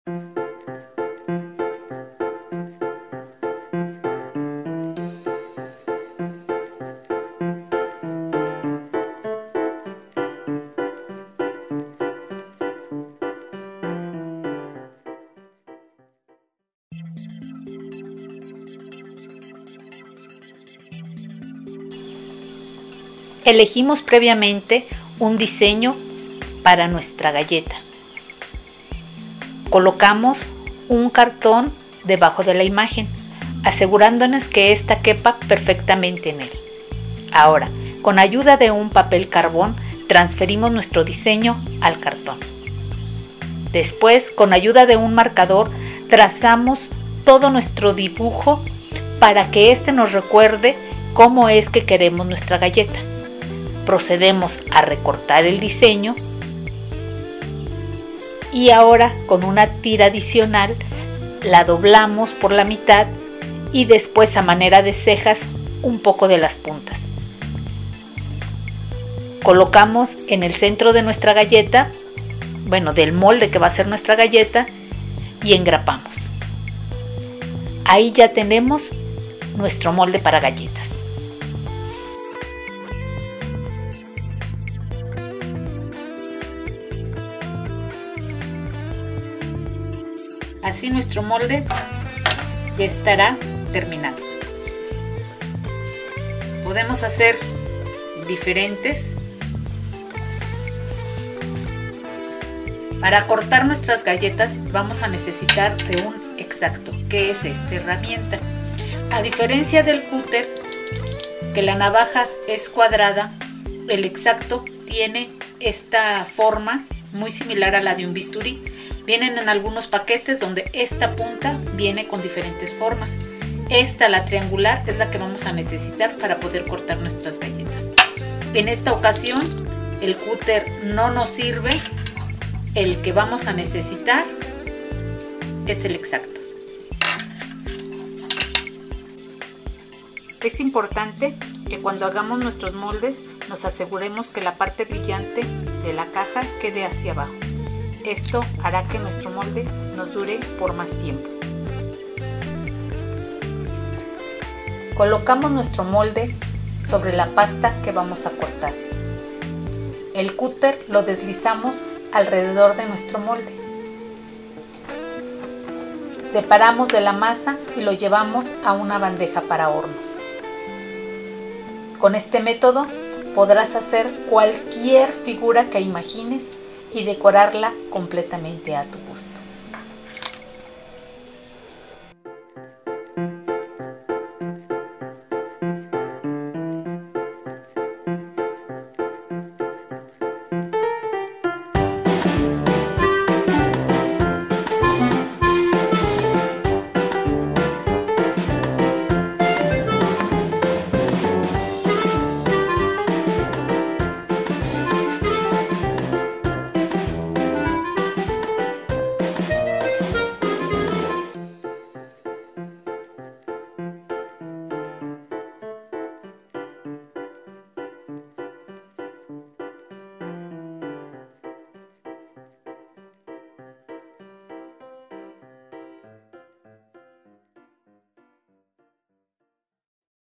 20 4 Cómo Hacer un Molde de Galletas Casero ☺☺
Te muestro como hacer y usar un molde para galletas casero. Muy practico, económico y fácil de usar.
En la pestaña de inicio/comunidad encontraras información muy útil. No dejes de visitarla.